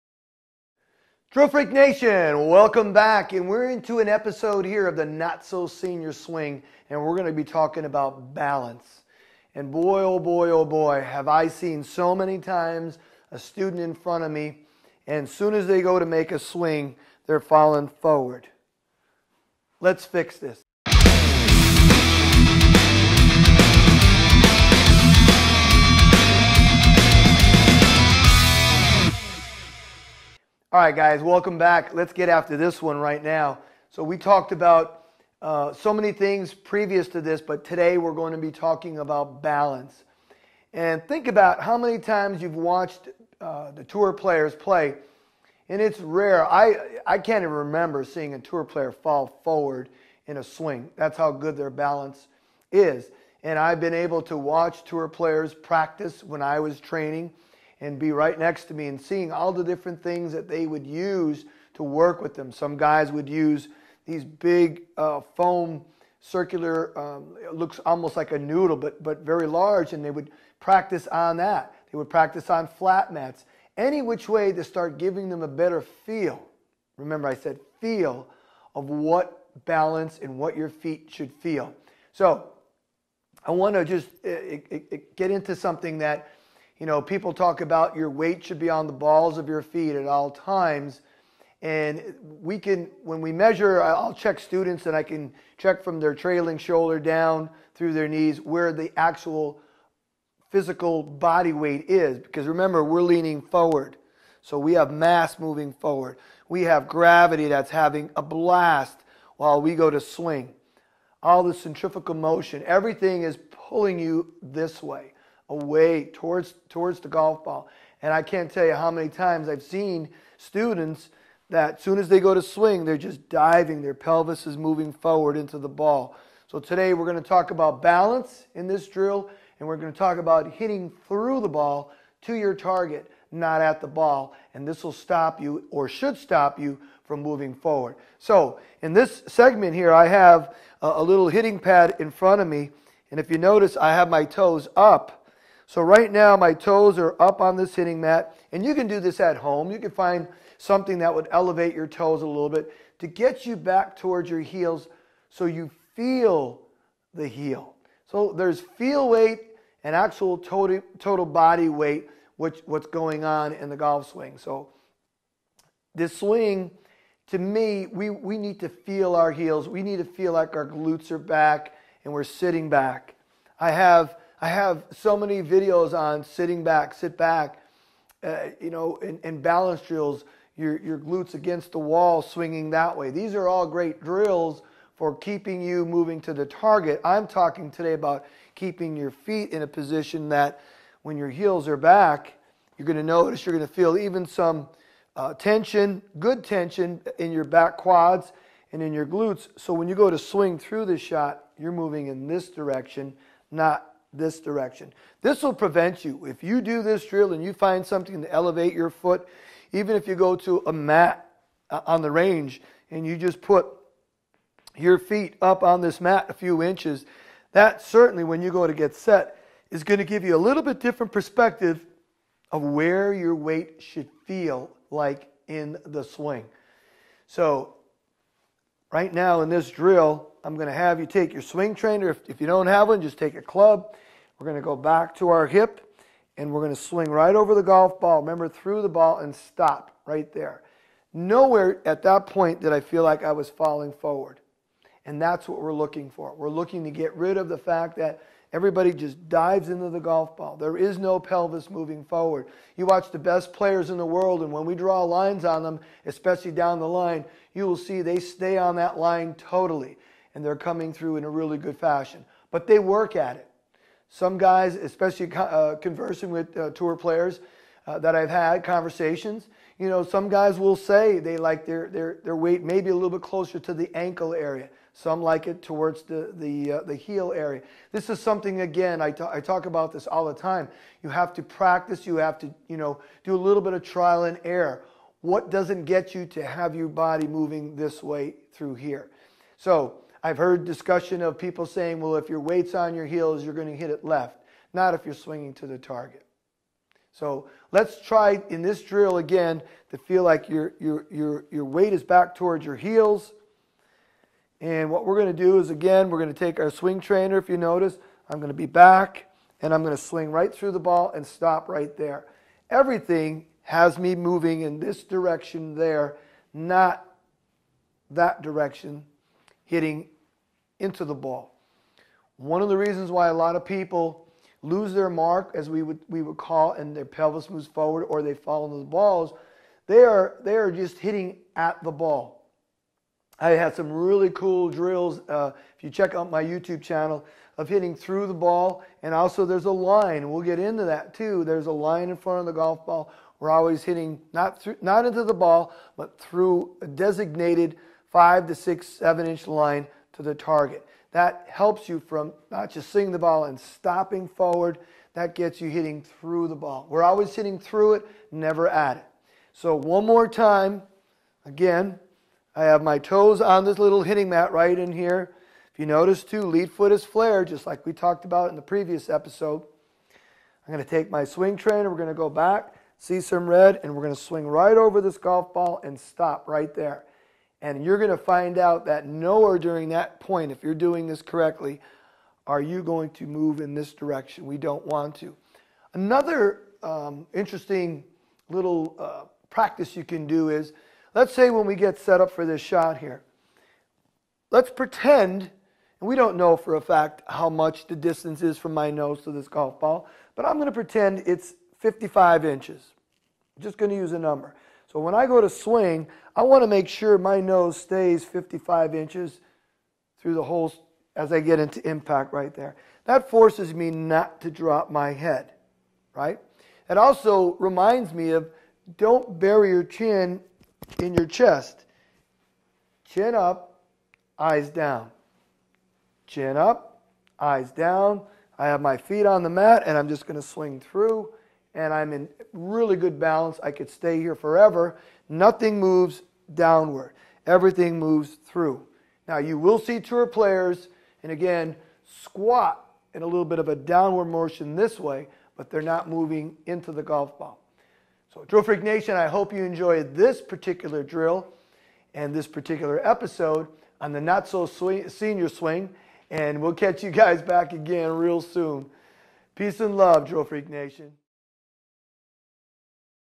The Not-so-Senior Swing Episode 4 Balance
The NOT-so-Senior Swing Series consists of great practice drills to get you in better positions in your golf swing for increased clubhead speed and more consistent shots.
In episode 4 of our series, you’ll learn how to keep your balance in your swing to create better shots – for seniors … but not just for seniors!
0:00 Intro
0:30 How to Keep your Balance in the Golf Swing
2:20 Balance Drill
4:57 Repeat of the Balance Drill